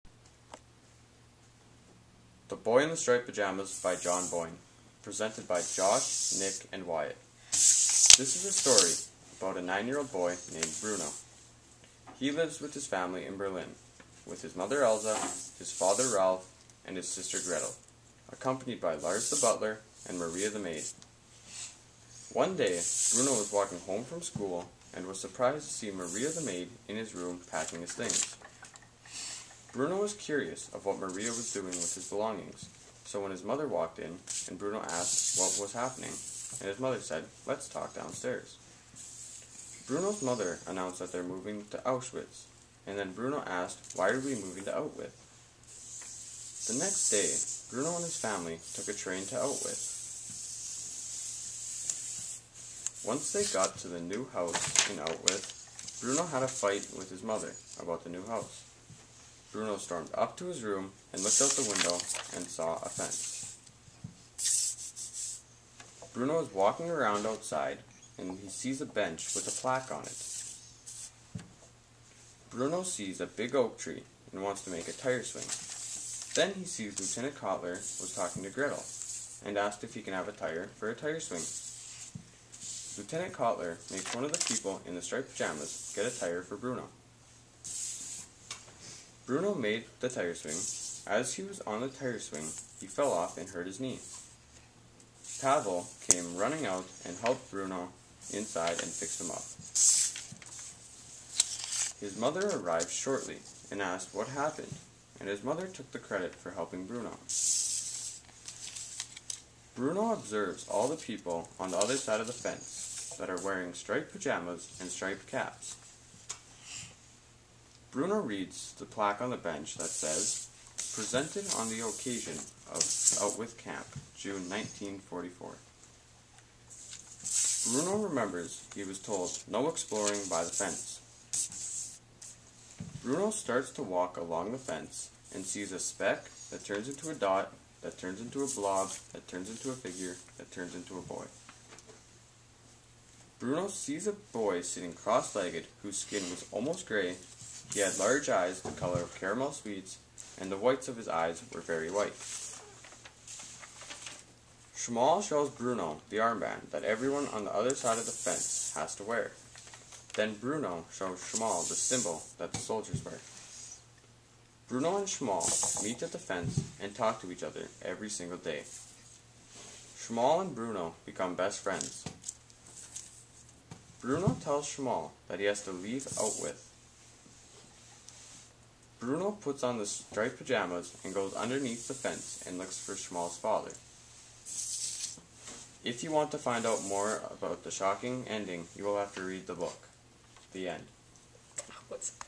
Common Craft-style Book Trailer - The Boy in the Striped Pajamas (by Josh, Nick & Wyatt)
Common Craft-style Book Trailer - permissions granted by Common Craft for our Grade 8 class to create book trailers in 'Common Craft style'.